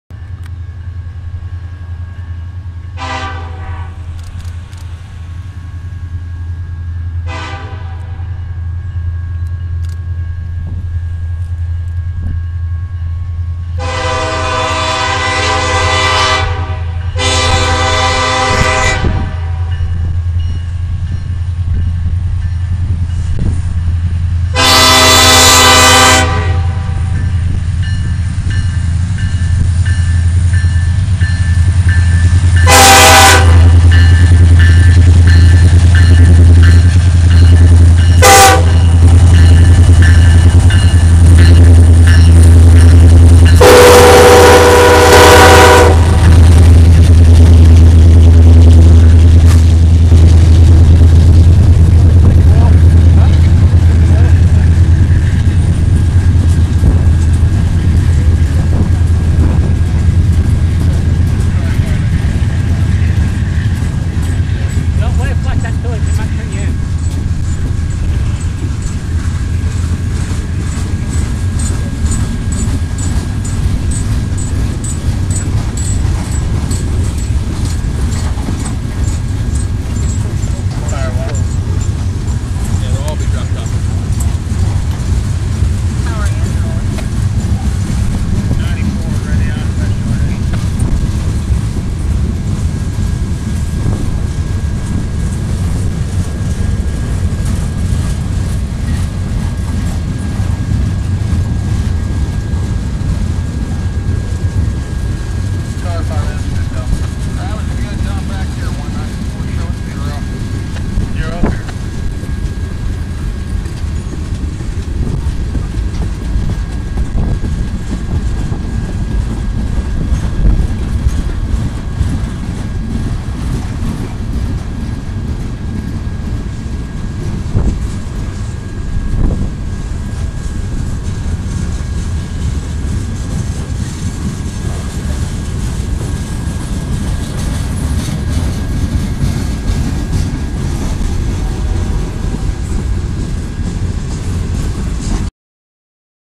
Railfanning Bellevue, Ohio. Freight Train 18M Leaving Town.
Spent this past weekend with Trey, Jordan, Josh, and Clint railfanning around Columbus and Bellevue, Ohio. On Saturday, we spent several hours at Bellevue watching trains come in and out of the yard. A few minutes after 18N had arrived into Bellevue Yard. It was Eastbound freight train 18M's turn to leave town and head south on the Sandusky District toward Columbus with SD70M-2 2730 leading the way. If you look in the background, you can see the tail end of 18N waiting to be brought into the yard.